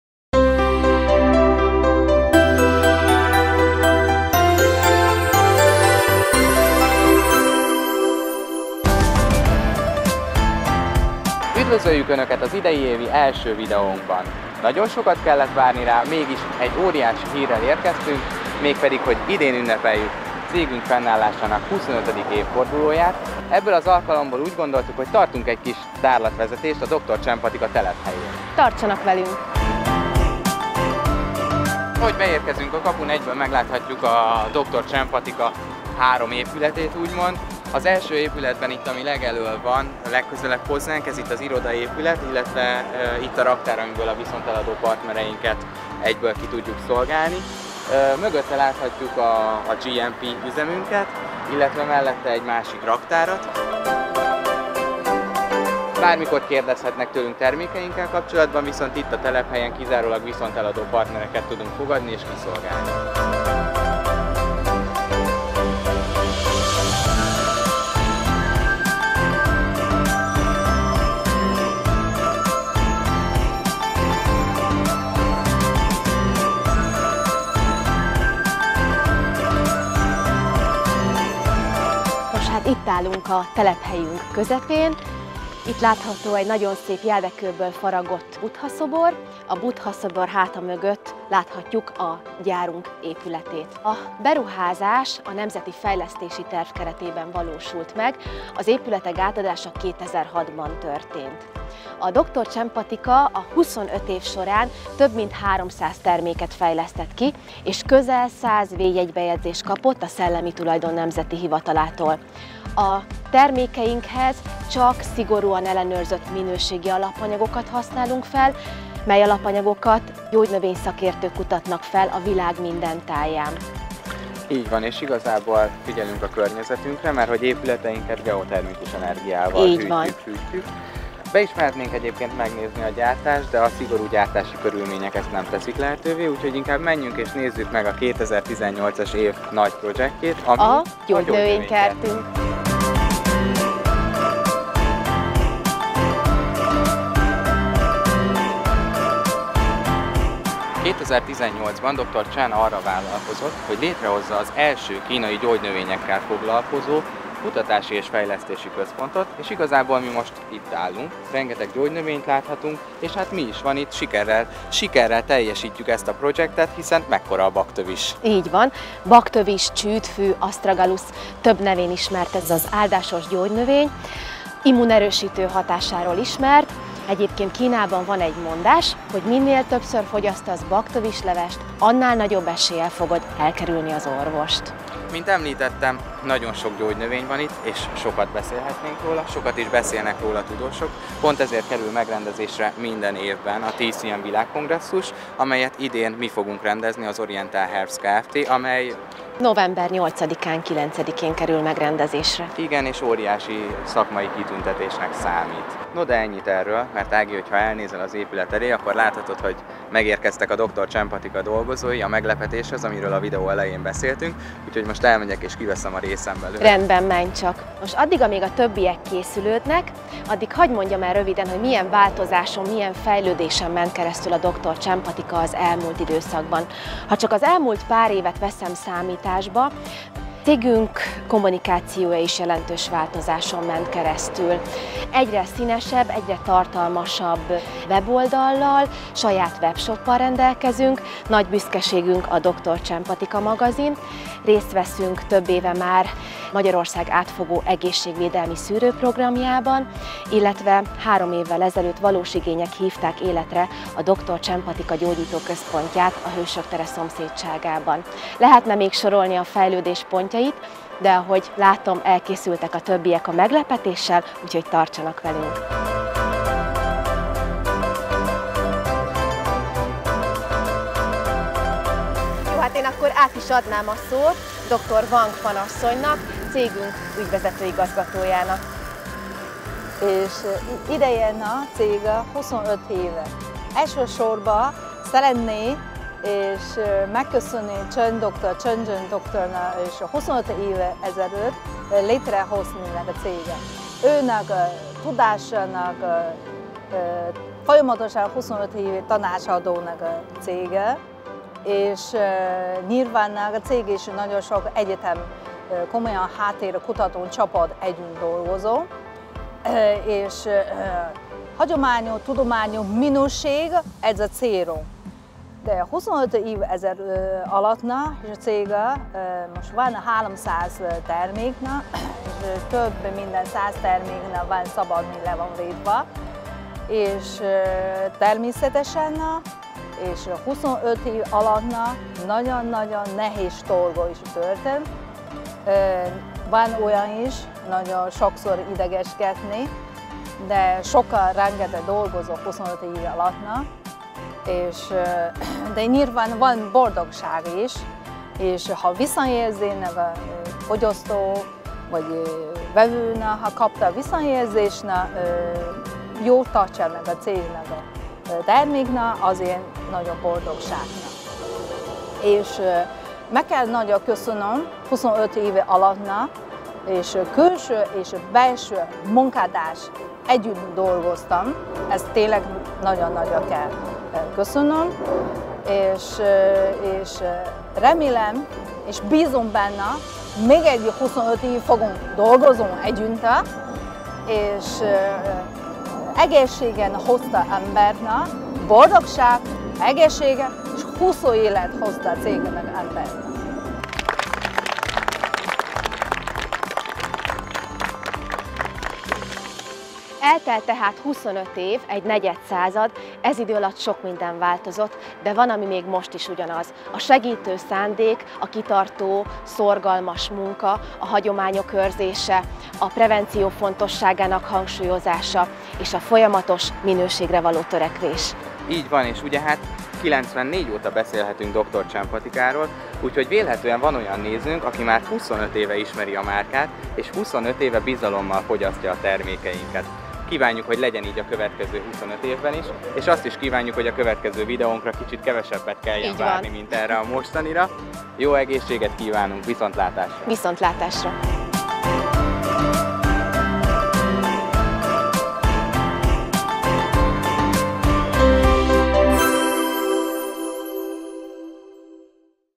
Negyed század az egészség szolgálatában - Dr. Chen Patika
Immár 25 éve dolgozunk töretlenül az egészségért.  - A negyed évszázad alatt cégünk rengeteg változáson ment keresztül, de ami fontos, hogy cél ugyanaz maradt. Dr. Chen Patika - Hagyomány. Tudomány. Minőség.